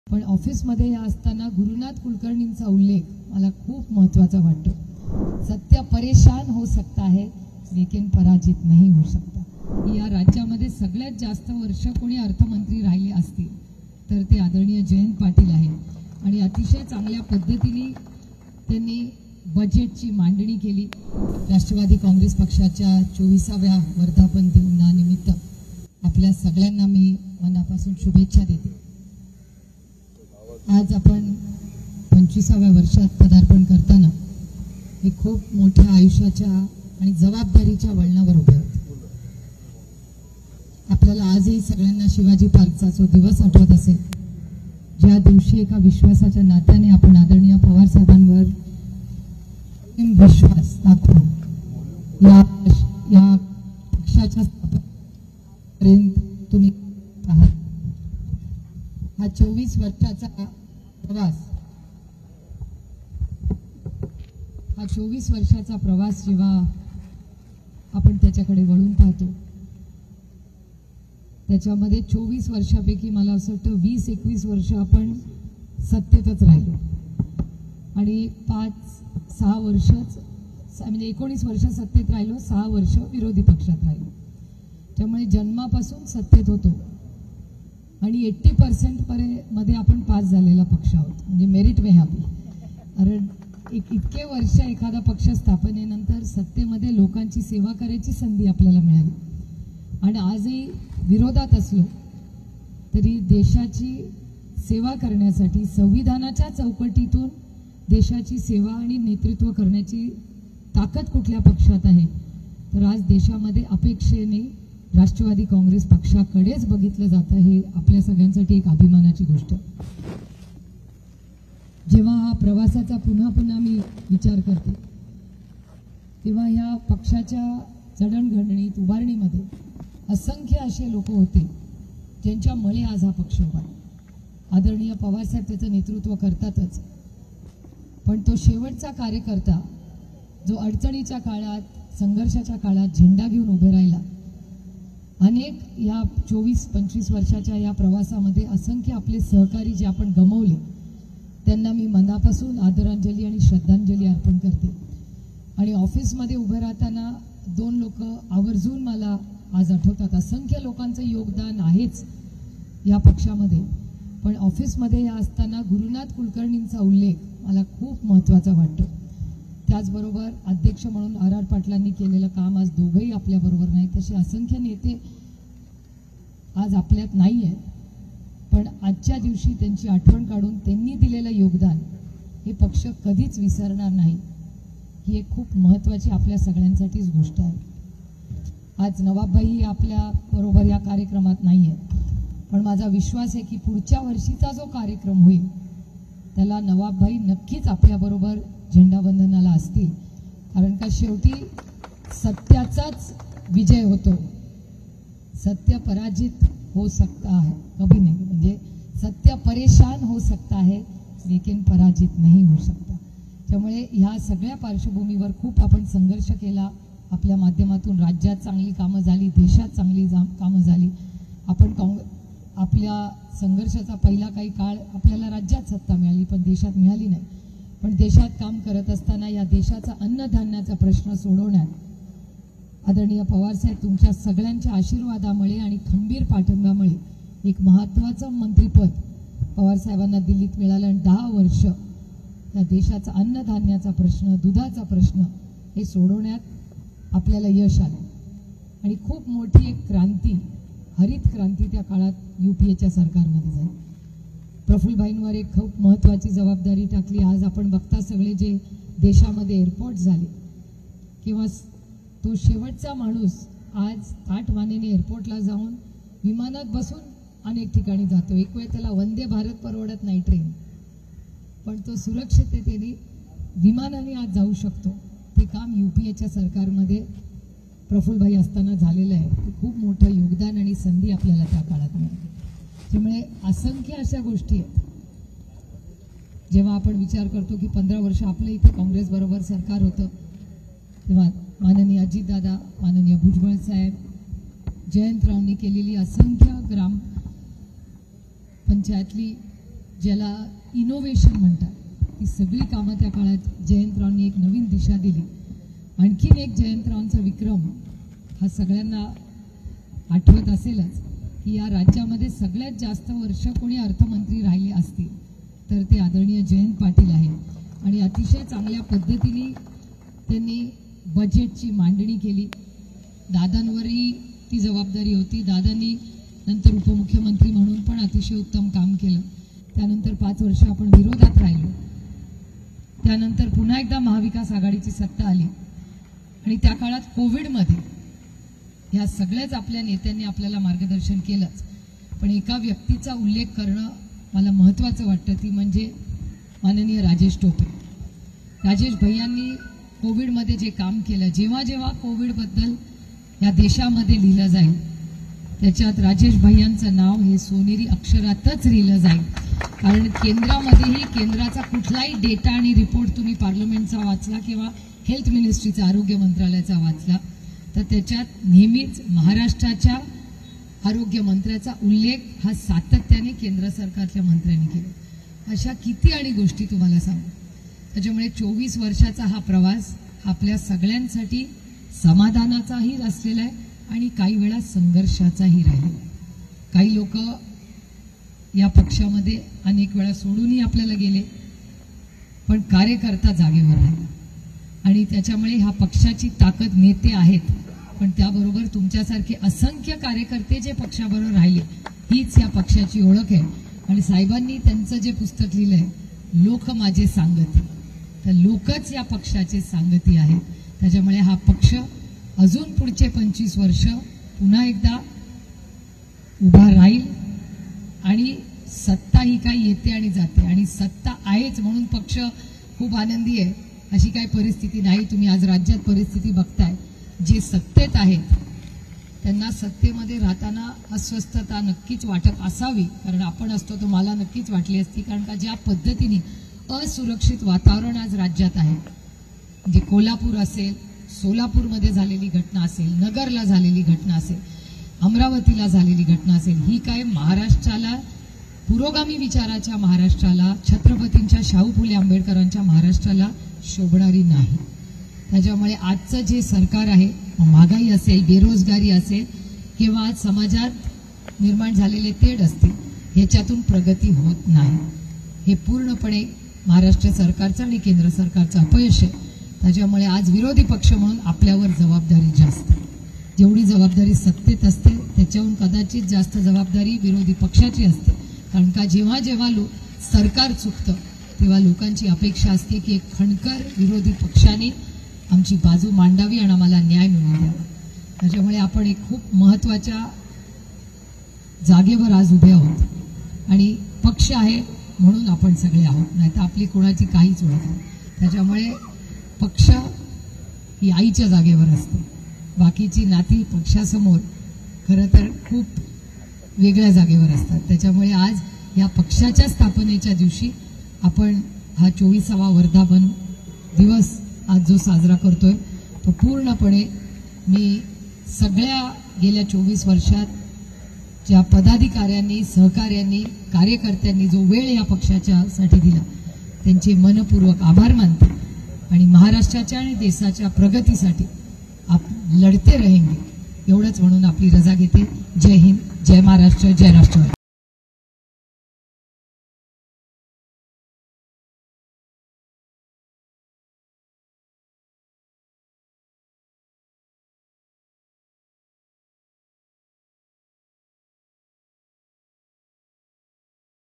Supriya Sule | पक्षातील नेत्यांचं कौतुक करताना सुळे थकल्या नाही... | LetsUpp Marathi
राष्ट्रवादी काँग्रेस पक्ष पंचविसाव्या वर्षात पदार्पण करत आहे. वर्धापनदिनानिमित्त पक्षातर्फे आयोजित करण्यात आलेल्या कार्यक्रमात खासदार सुप्रिया सुळे यांनी पक्षाच्या 24 वर्षांतील वाटचालीचा आढावा घेतला.